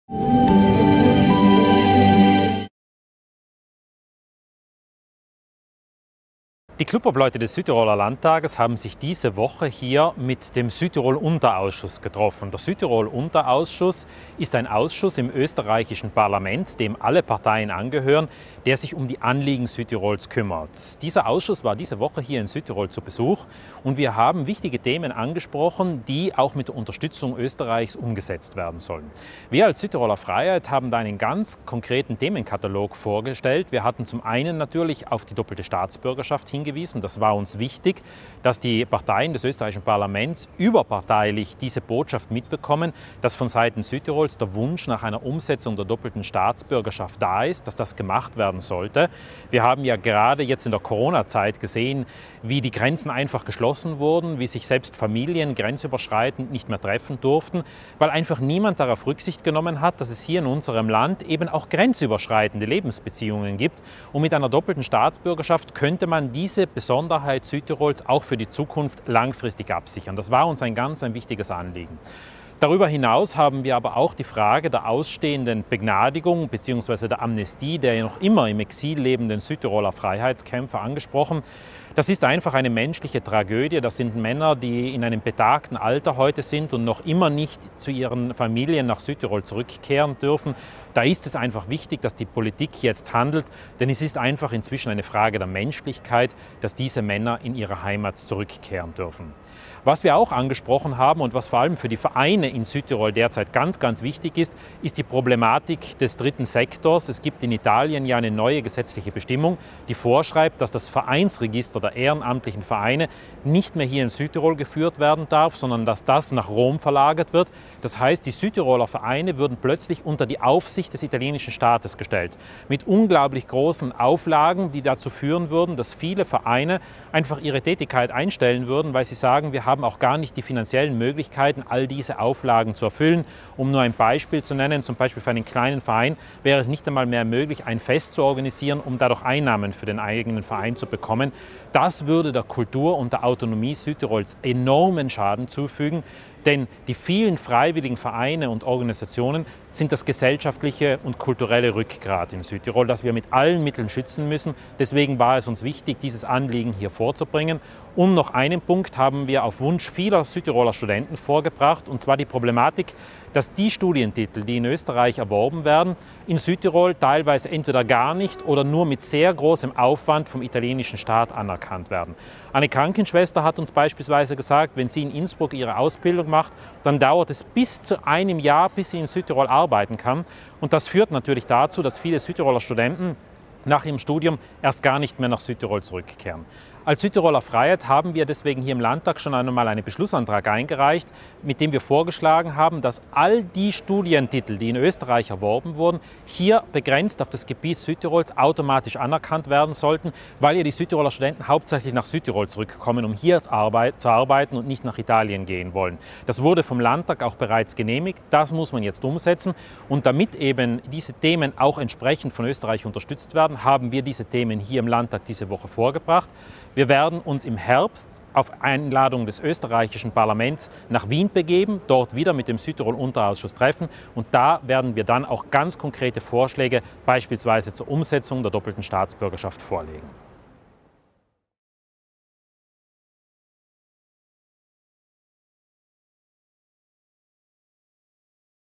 Von Doppelpass bis Ehrenamt: Vertreter Österreichs in Süd-Tirol
Diese Woche waren Vertreter des Vaterlandes Österreich zu Gast in Süd-Tirol. Mit den Mitgliedern des Süd-Tirol Unterausschusses des österreichischen Parlaments wurden zahlreiche wichtige Themen besprochen, die mithilfe Österreichs umgesetzt werden sollen. Zur Sprache kamen dabei die doppelte Staatsbürgerschaft, die Anerkennung von Studientiteln, die Begnadigung der noch im Exil lebenden Freiheitskämpfer und die Angriffe Roms auf das Ehrenamt in Süd-Tirol.

Der Landtagsabgeordnete der Süd-Tiroler Freiheit, Sven Knoll, blickte auf das Treffen zurück und auf die nächsten Schritte voraus.